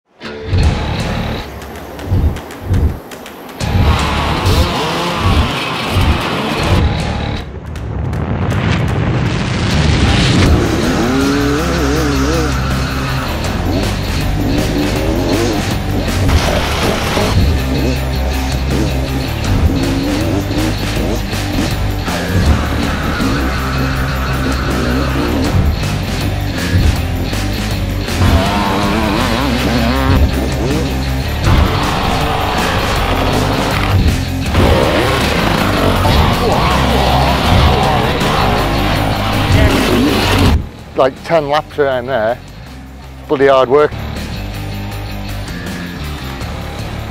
THE TOUGH 100 - 2018 - DAVID KNIGHT - TRAILER
The Tough 100!

What an event today!

Here's our short trailer for the video to follow.
Well done to all the riders who entered a 5 hour race in the blistering heat!

WOR Events
CF Racing
Michelin